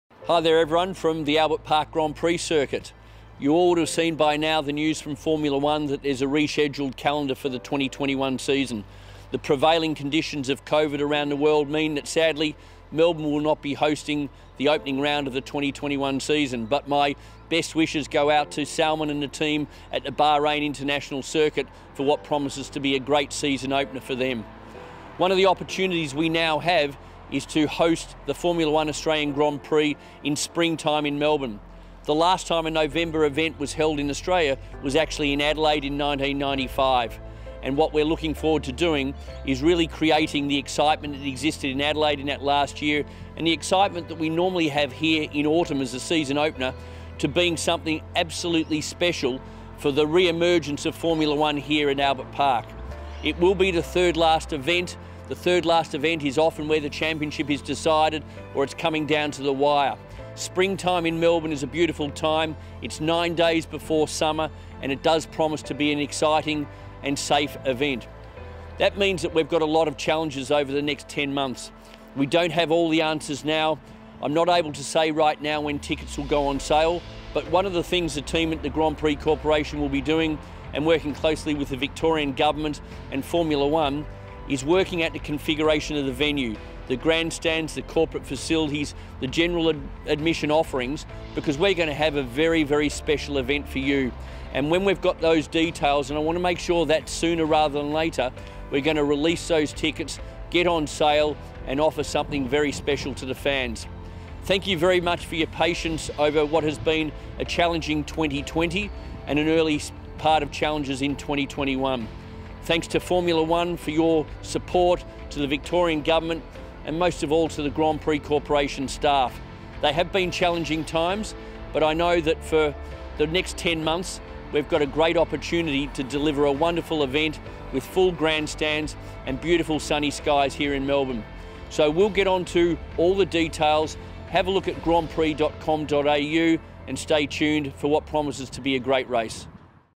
Australian Grand Prix 2021 Postponed, a message from the CEO
Thank you Australian Grand Prix fans, for your patience and understanding.

Australian Grand Prix Corporation CEO Andrew Westacott delivers a message to Australian Grand Prix fans following the news that the 2021 race has been postponed until November.